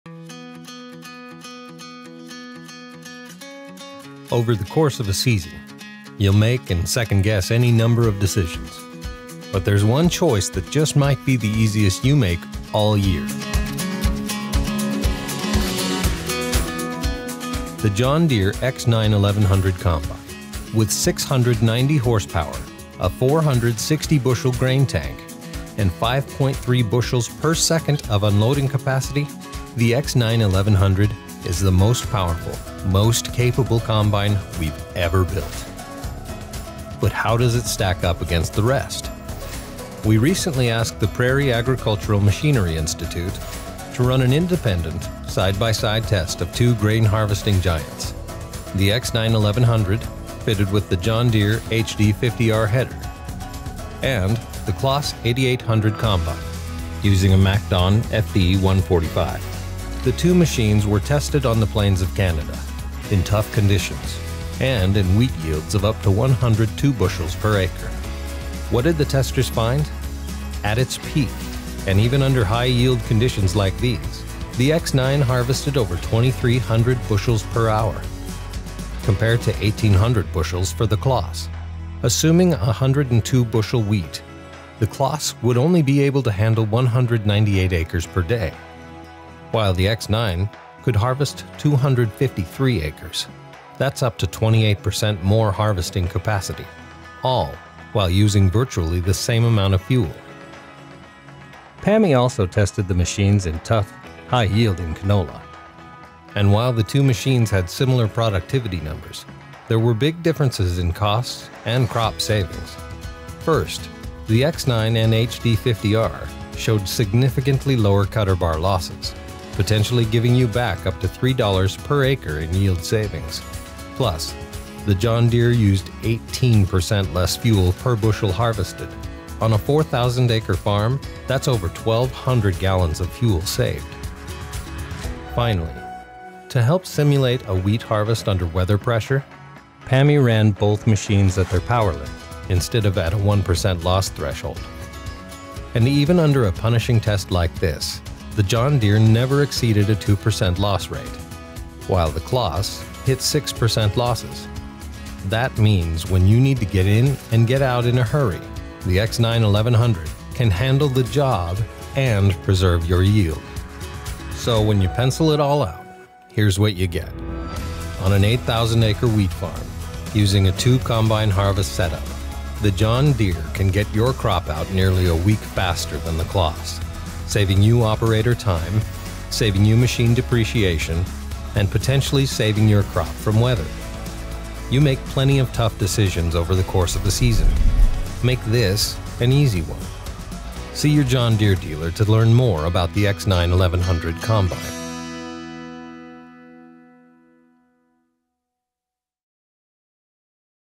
+Gain 7 Days at Wheat Harvest | John Deere
According to independent testing by the Prairie Agricultural Machinery Institute (PAMI), the X9 1100 with HD50R header delivered 28% more harvesting capacity with no sacrifice in grain quality when compared with equipment from other manufacturers. That means you can finish wheat harvest a week earlier and move on to other crops. In canola, this combine and header also captures more crop, providing an additional $3.00 per acre of potential revenue.